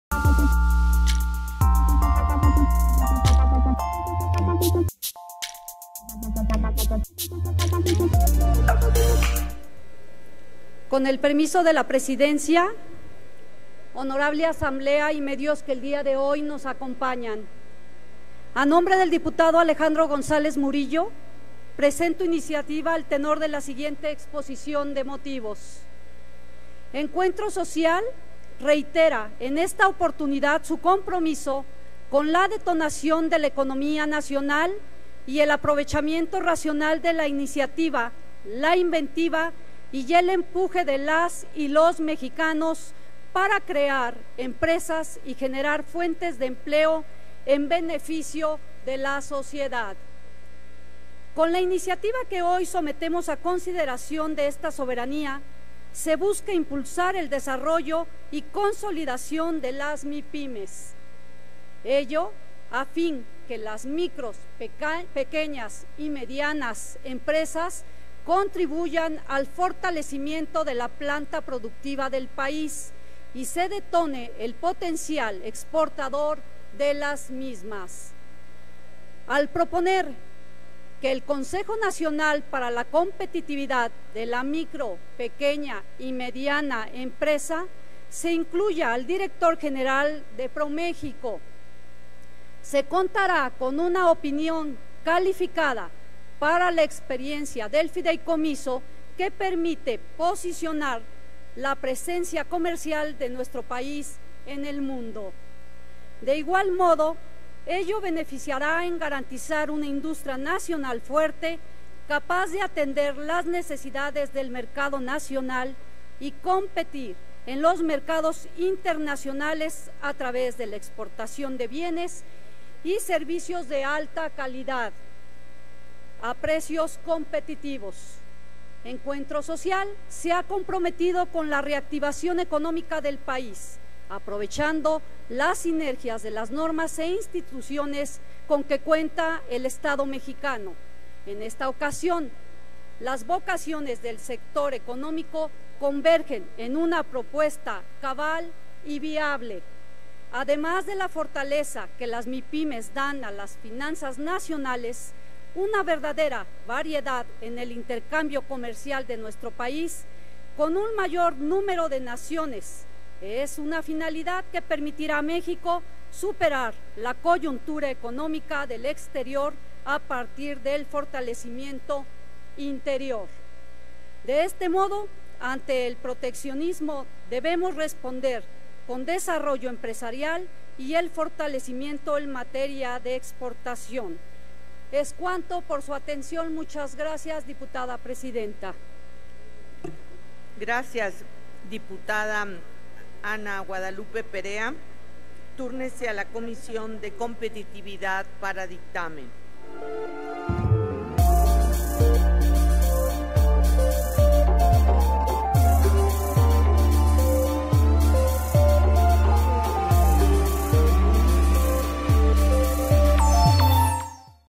Dip. Ana Perea (PES) - Iniciativa
La iniciativa de la Dip. Ana Guadalupe Perea Santos (PES) tiene por objeto fomentar la participación de ProMéxico en el desarrollo de las MIPYMES.